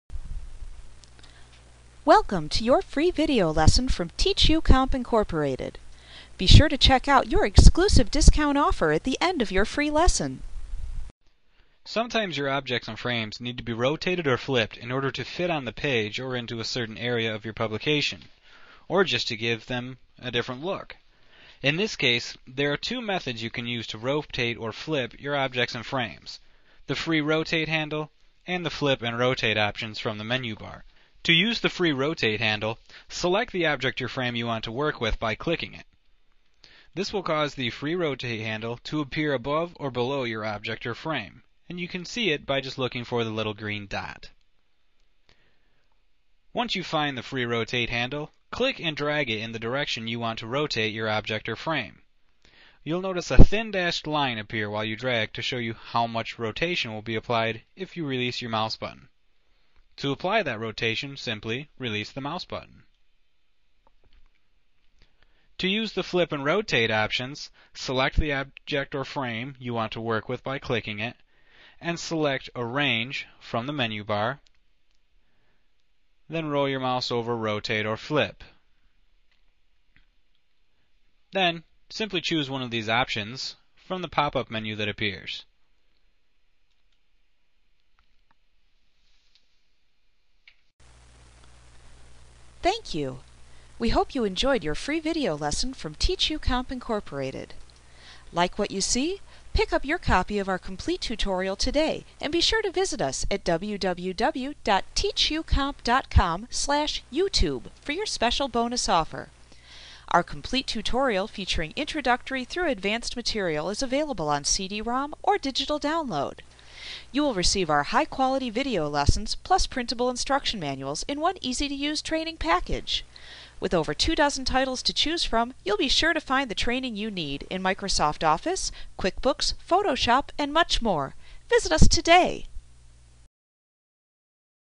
Publisher 2003 Tutorial Rotating and Flipping Objects Microsoft Training Lesson 3.8
Learn how to rotate or flip an object in Microsoft Publisher A clip from Mastering Publisher Made Easy v. 2003. - the most comprehensive Publisher tutorial available. Visit us today!